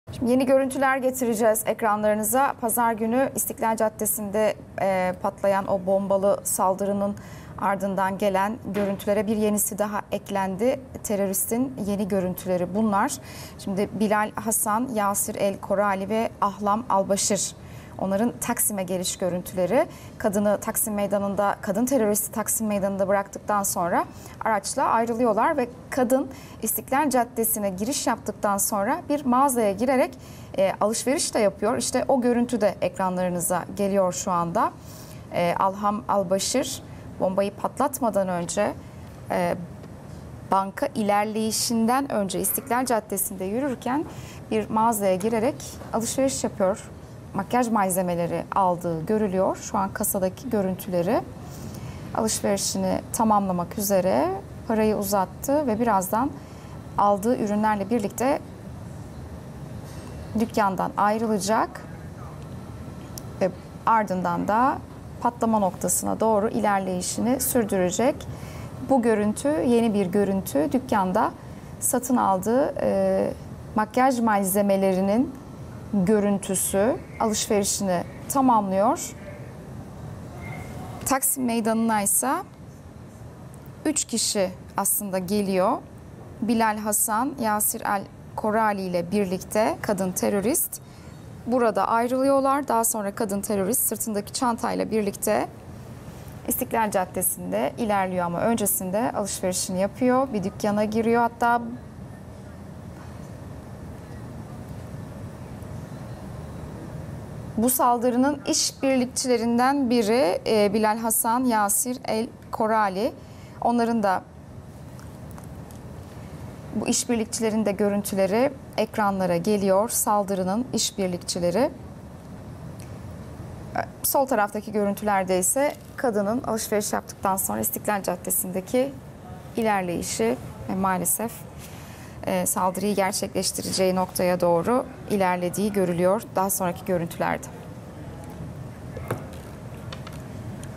Teröristin makyaj malzemesi aldığı görüntüler ortaya çıktı
Pazar günü İstiklal Caddesi'nde patlayan bombalı saldırının ardından gelen görüntülere bir yenisi daha eklendi.